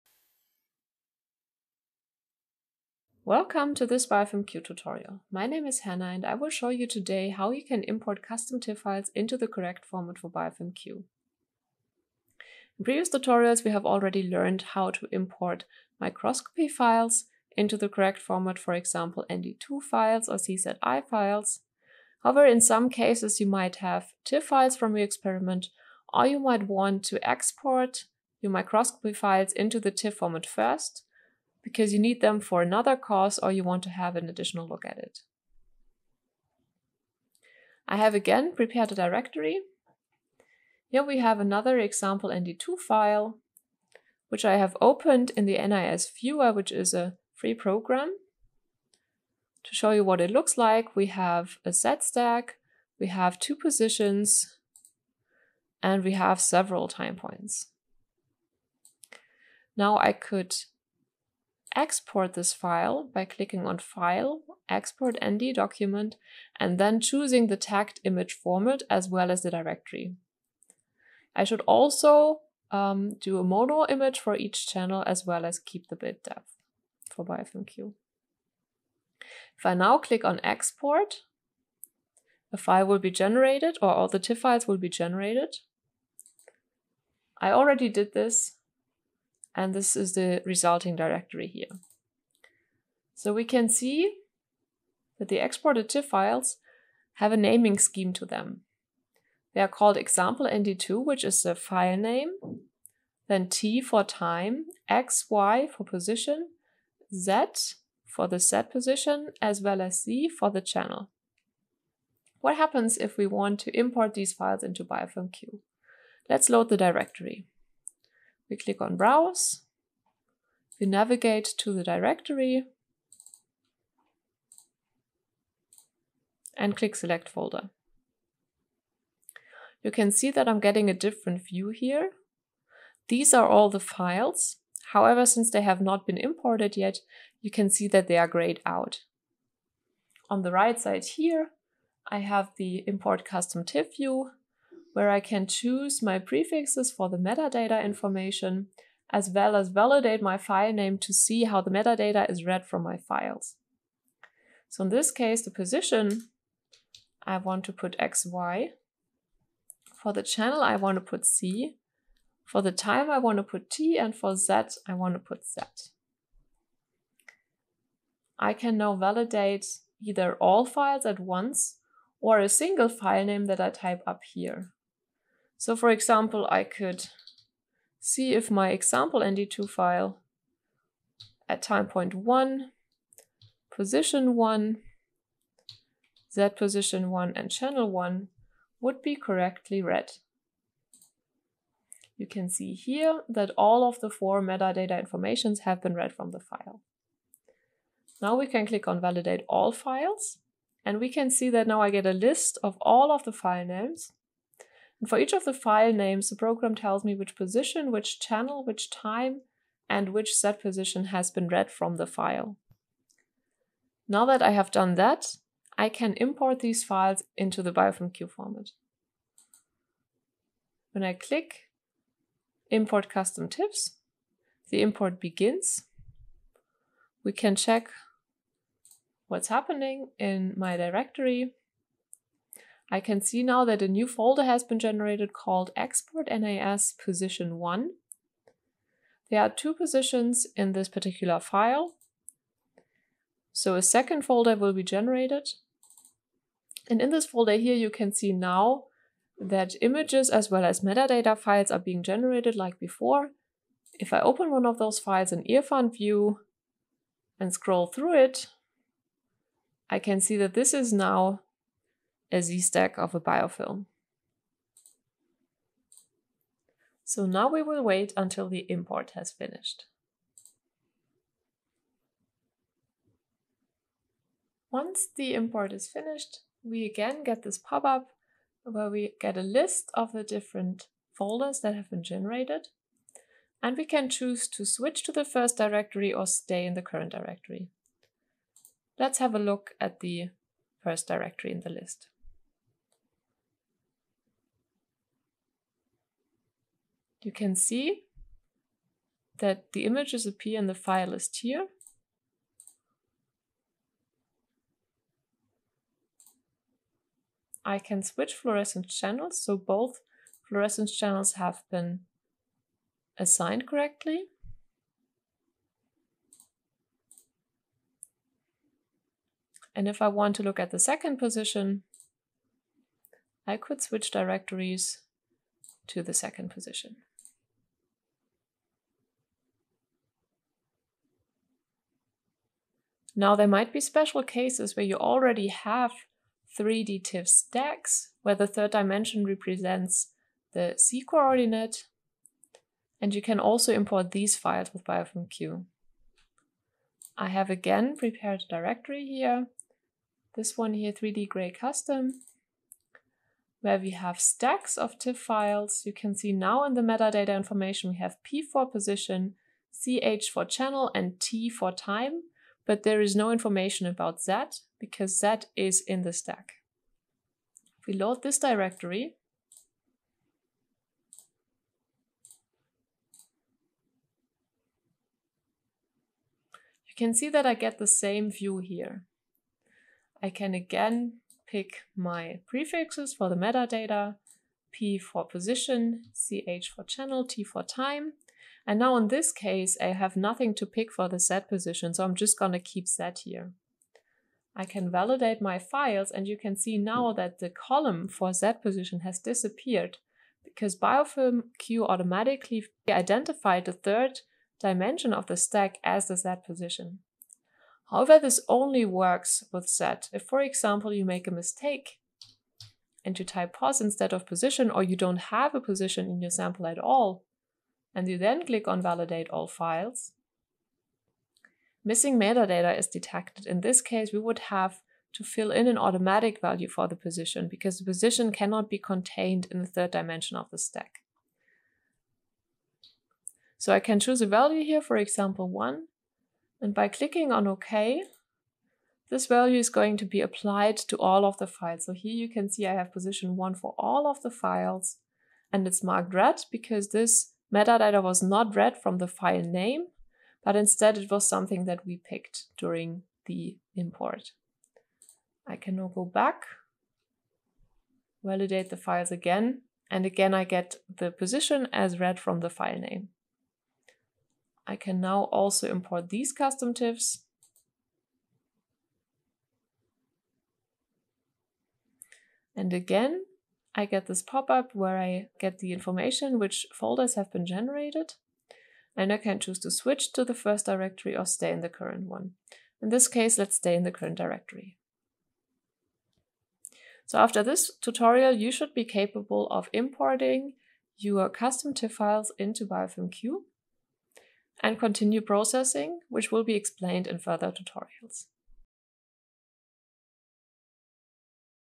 BiofilmQ Tutorials - 1.3.2 Import custom TIF files
BiofilmQ is a software tool for quantitative image analysis of microbial biofilm communities
BiofilmQ was developed by Raimo Hartmann, Hannah Jeckel, Eric Jelli, and Knut Drescher

Video was produced & edited by Martin Abt